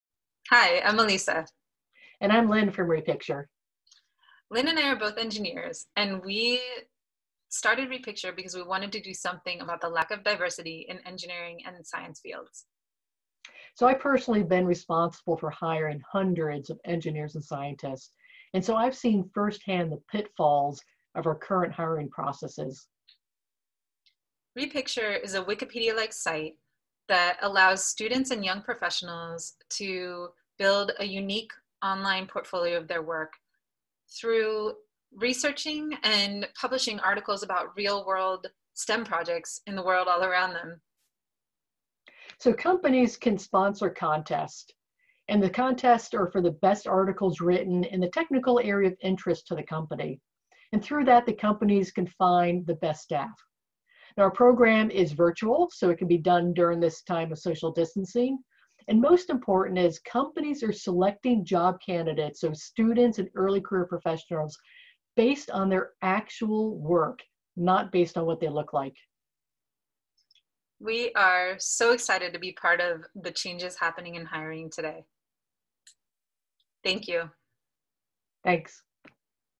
RePicture Co-Founders Introduce New Way For Companies to Find Staff
RePicture Co-Founders Aelisa and Lynn talk about a new way for companies to find the best staff to meet their science, technology, engineering, and math (STEM needs)